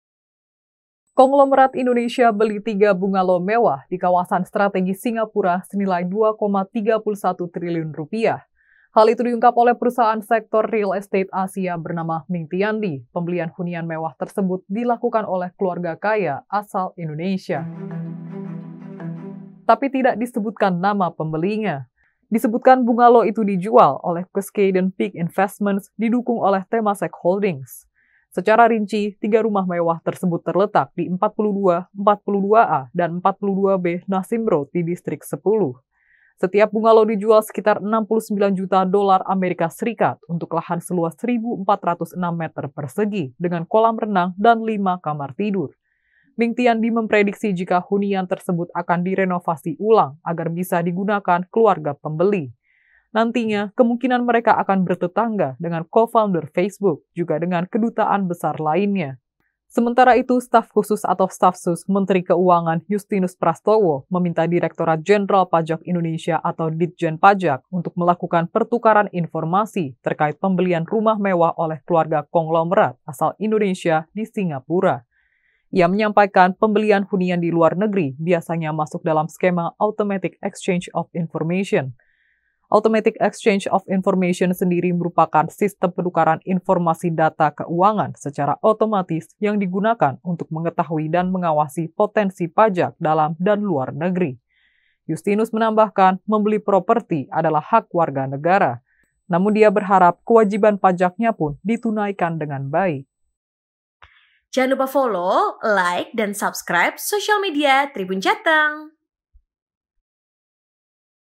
Viral Crazy Rich Indonesia Beli 3 Rumah Rp 2,3 T di Singapura
Konglomerat Indonesia beli tiga bungalo mewah di kawasan strategis Singapura senilai Rp2,31 Triliun (kurs Rp14.941)

Hal itu diungkap oleh perusahaan sektor real estat Asia bernama Mingtiandi, pembelian hunian mewah tersebut dilakukan oleh keluarga kaya asal Indonesia.

Tapi tidak disebutkan nama pembelinya.